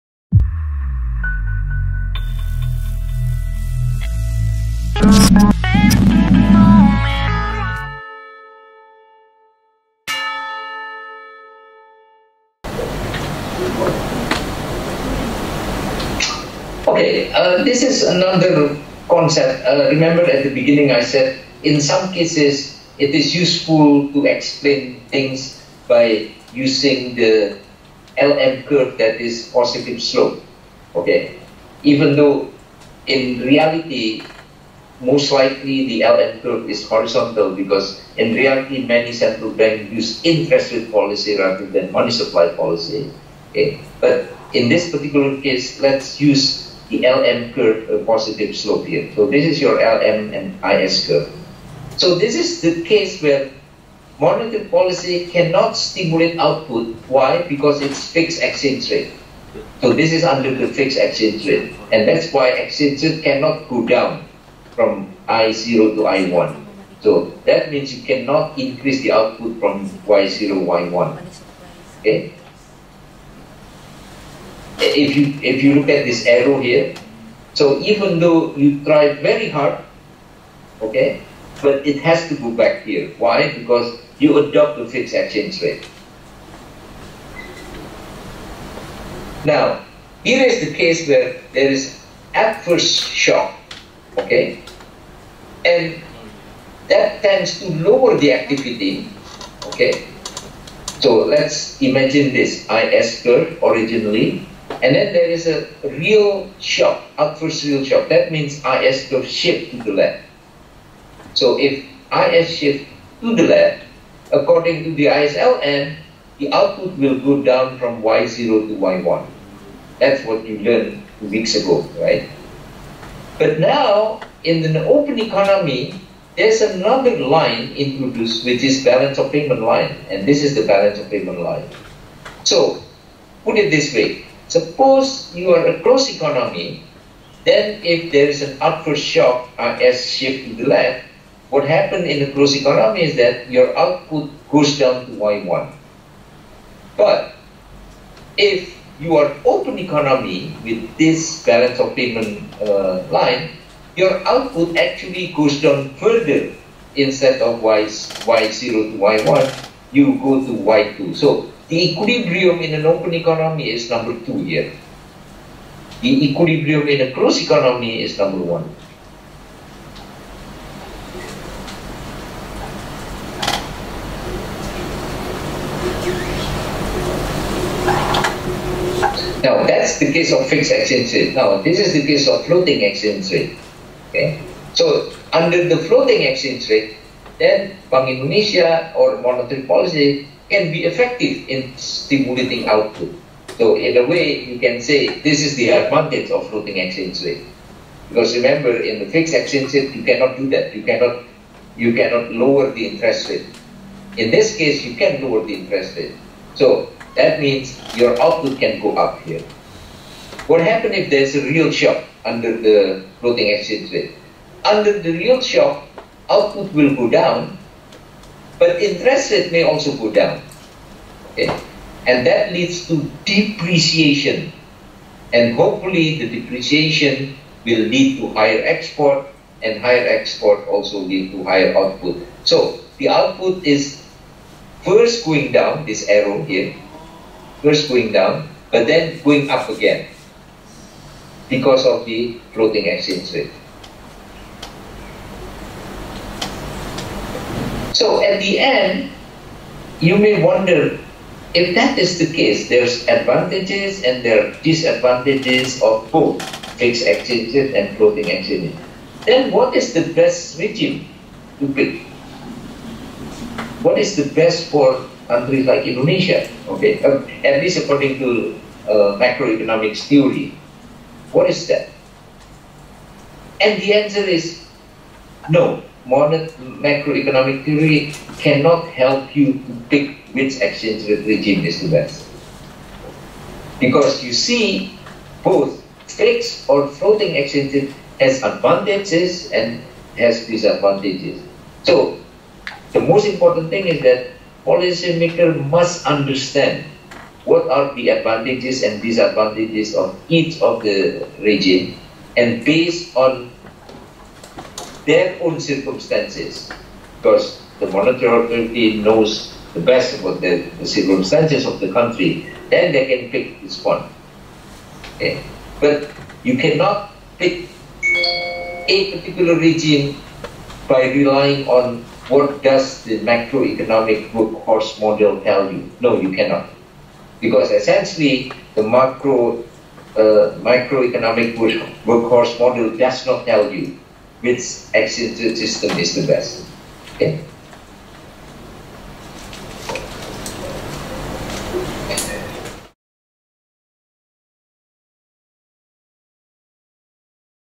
Macroeconomics 3: Exchange Rate Regimes, Monetary Policy, Fiscal Policy, & Output
Series Macroeconomics 3 udah sampai part kesepuluh aja nih, Sobat UI.

Kali ini kita belajar soal kebijakan moneter, yuk. Sobat UI udah tau belum apa kebijakan moneter itu? Kebijakan moneter adalah kebijakan yang dikeluarkan oleh bank sentral untuk mengatur persediaan uang di suatu negara. Emang kenapa sih uang aja koo diatur-atur?

Nah, cari jawabannya di video ini bareng Prof. Iwan J. Azis, Ph.D. ya😉

Open Videos (OVIS) Universitas Indonesia